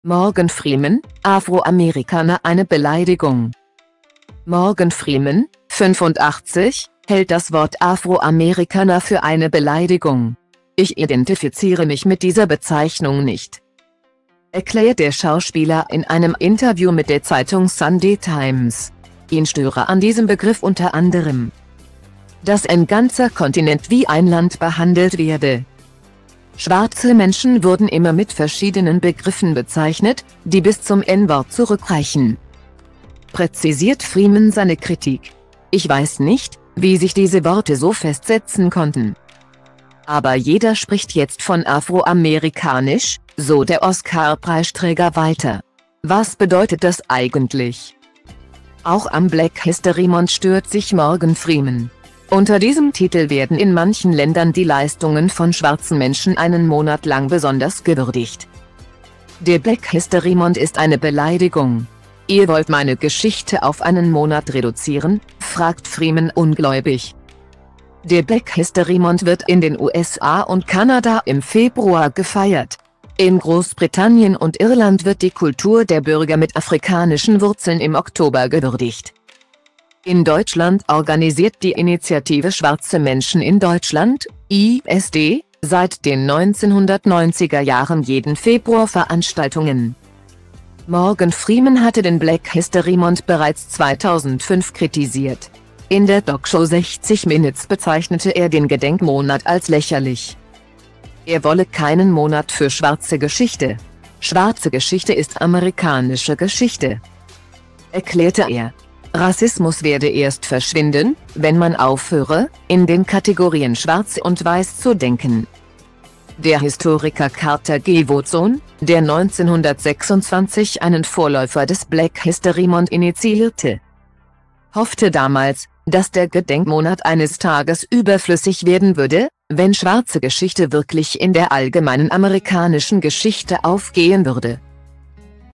Morgan Freeman: "Afroamerikaner" eine "Beleidigung"
Morgan Freeman (85) hält das Wort "Afroamerikaner" für eine "Beleidigung". "Ich identifiziere mich mit dieser Bezeichnung nicht",erklärt der Schauspieler in einem Interview mit der Zeitung "Sunday Times". Ihn störe an diesem Begriff unter anderem,dass ein ganzer Kontinent wie ein Land behandelt werde.